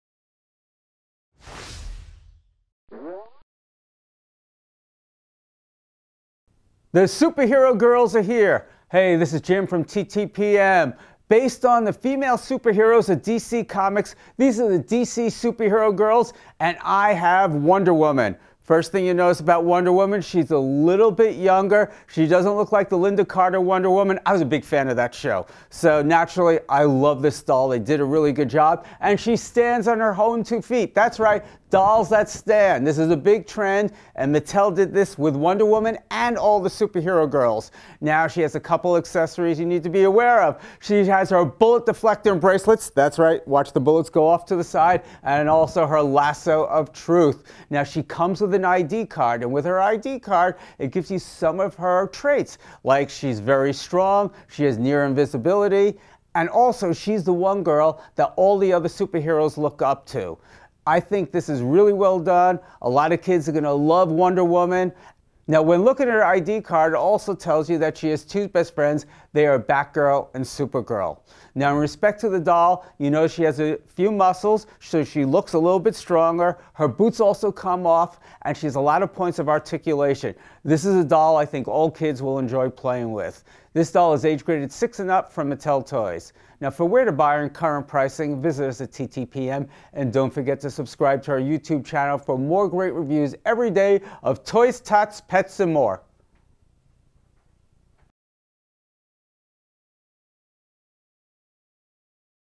DC Super Hero Girls Wonder Woman from Mattel
DC Super Hero Girls Wonder Woman action doll from Mattel. Teen version of classic DC Comics character. Wears sparkly red top with Wonder Woman logo collar, blue pants, gold headpiece, bullet-deflecting bracelets, and red boots. Comes with lasso of truth accessory. Poseable doll is 12-inches tall and stands on its own. Part of a line that also includes Supergirl, Harley Quinn, Batgirl, Bumblebee, and Poison Ivy.

Product Info:
The DC Super Hero Girls Wonder Woman action doll is a teenaged version of the classic DC Comics character wearing a sparkly top with Wonder Woman logo collar, blue pants, and red boots.